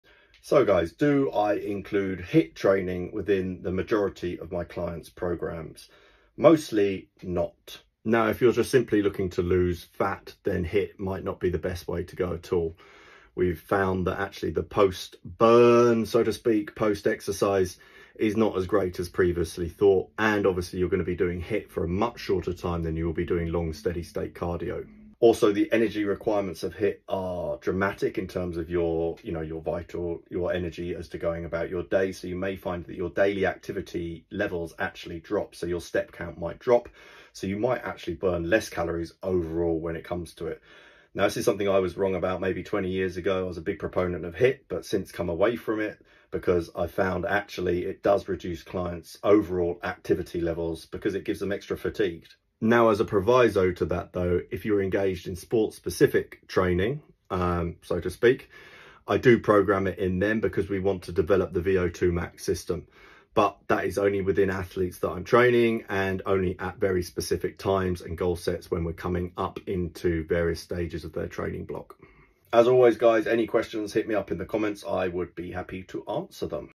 WHY I DON’T INCLUDE HIIT TRAINING .. and maybe you shouldn’t .
The reason why I don’t include hiit training with majority of clients . If you need help with fat loss , training and generally being stronger and healthier hit me up